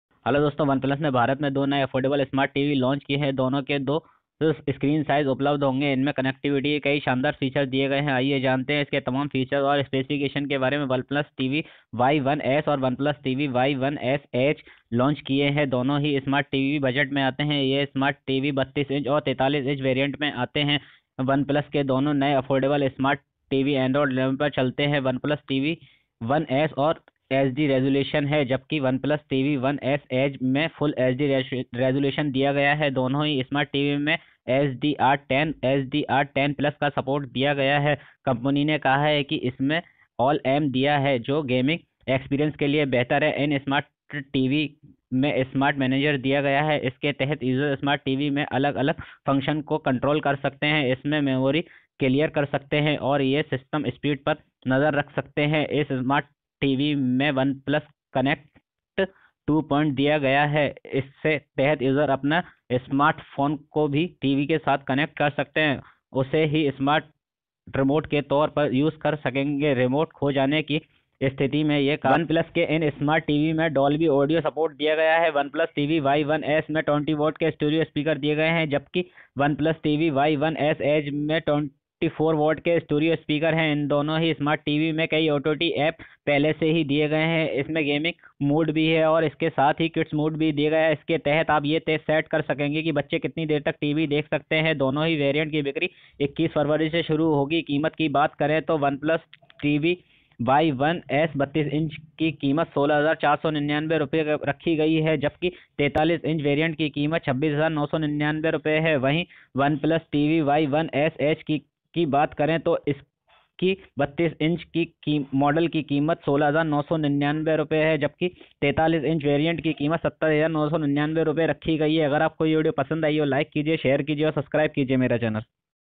oneplus# OnePlus TV Y1S & Y1S Edge | OnePlus TV with Stereo Speaker, kids mode, gaming mode...
hello dosto
onepluse ne aapne 2 new tv launch kar diye hai oneplus tv y1s aur oneplus tv y1s edge ye dono hi tv 21 February sale ke liye available honge is me aapko kids mood Milega jese aap ye set kar sakte hai ki aapke bachhe kitni der tak tv dekh sakenge aur ise aap apne mobile se bhi operate kar sakte hain isme dolby speaker Diye gaye hai ye tv aa sani se aapke budget me bhi fit ho sakte hai in budget tv ki starting price 16,499 rupees hai.